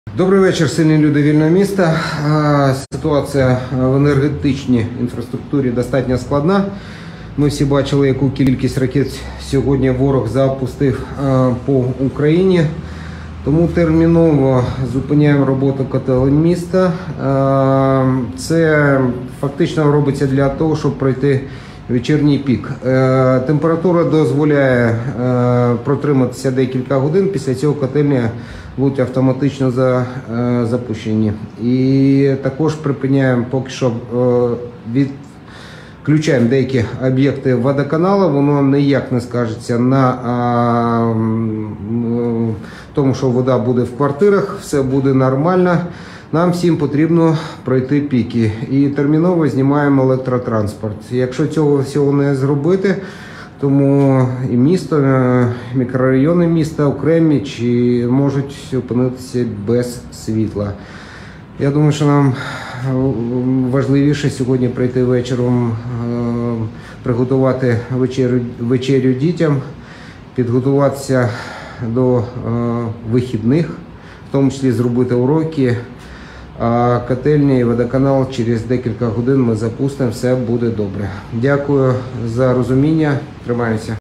Наслідки ракетного обстрілу: у Житомирі зупинили котельні, об’єкти водоканалу, електротранспорт
Сьогоднішній масований ракетний обстріл росії не минув безслідно для Житомира. Енергетична система міста не витримує навантаження. Робота котелень зупинена, попередньо на час вечірнього піку. Частково вимкнено деякі об’єкти водоканалу, однак на водопостачання в квартирах житомирян це не вплине. Окрім того терміново зупинено електротранспорт. Попередньо, завтра він також не вийде на маршрути. Про це повідомив міський голова Сергій Сухомлин у своєму вечірньому зверненні